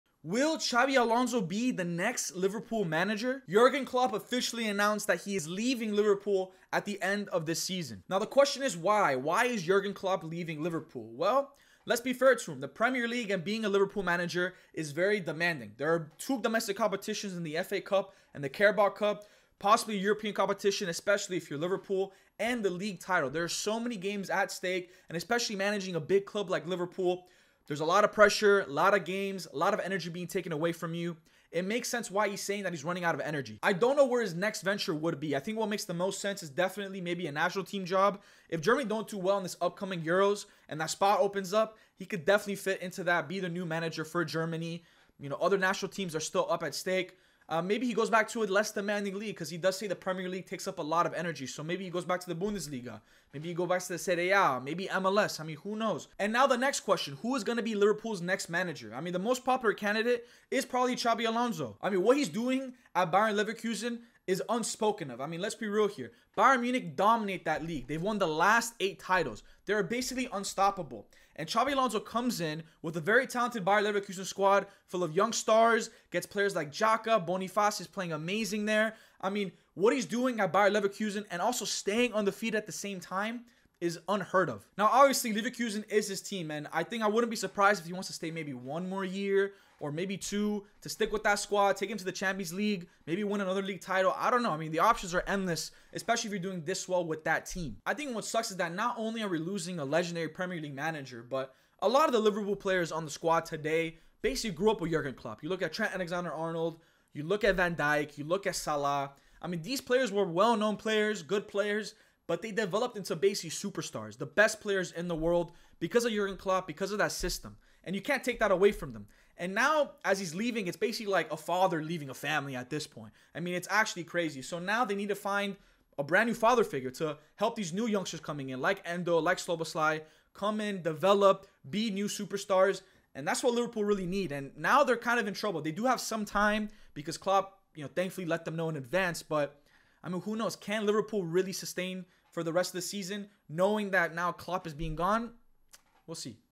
IS XABI ALONSO REPLACING JURGEN KLOPP AT LIVERPOOL???
End of an Era...